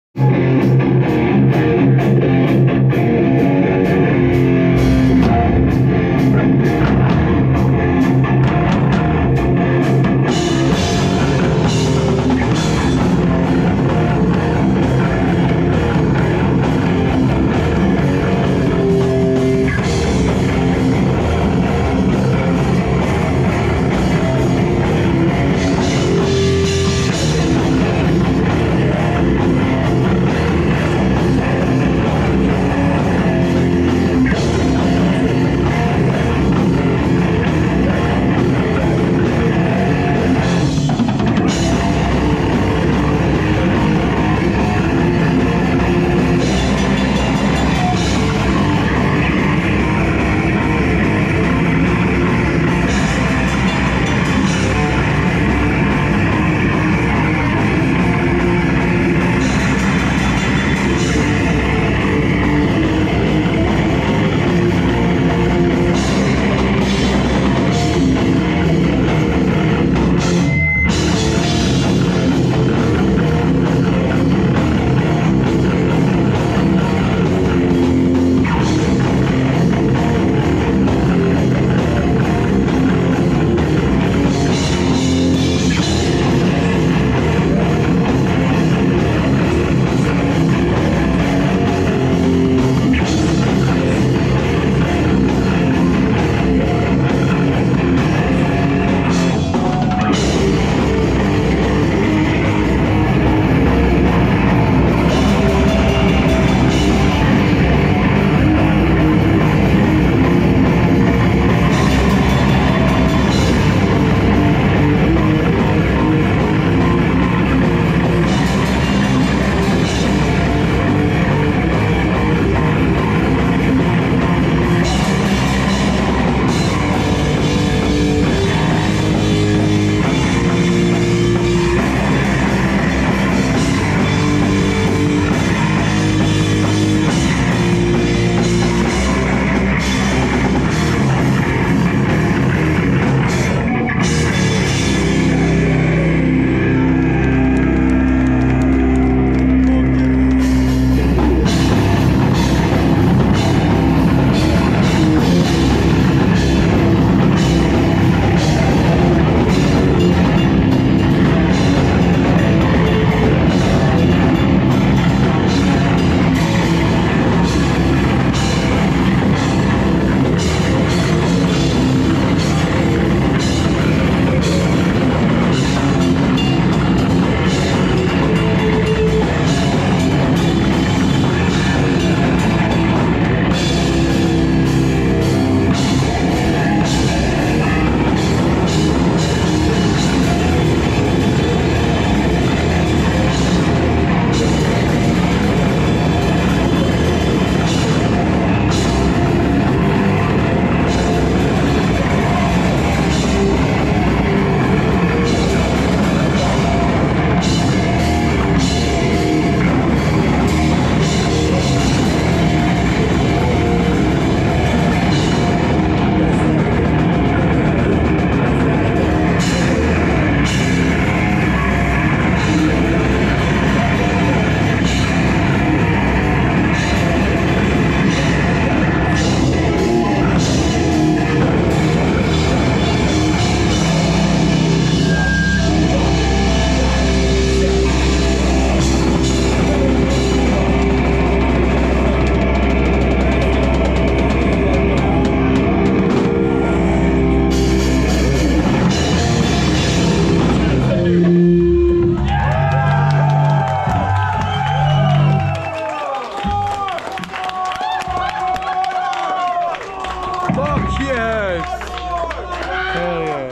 Unholy Grave (Japan) FULL-SET Part 4/4 LIVE @ Eagles Aerie 4-28-2024
The Legendary Unholy Grave comes all the way from Japan to play a few dates on the West Coast. This is their performance at Eagles Aerie in Henderson/Las Vegas, Nevada on April 28th 2024
With Support from: Acid Relapse, Gestation, Life's Torment, Law Abiding Citizen, and Illegal Aliens

PART4/4 FULL SET